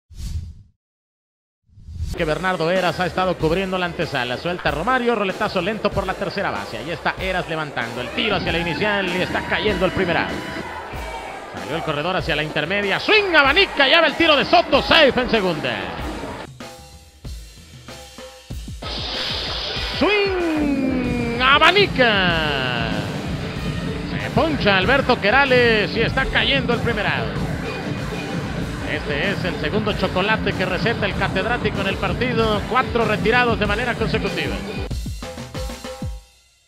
Joya de Romario Gil
Romario Gil consiguió su segunda victoria de la campaña en la mega paliza que los Pericos de Puebla le dieron a los Piratas de Campeche por score de 24-3. El zurdo culichi en su labor lanzó por espacio de 5 entradas en las que toleró 1 imparable y 1 carrera, una base y 4 chocolates.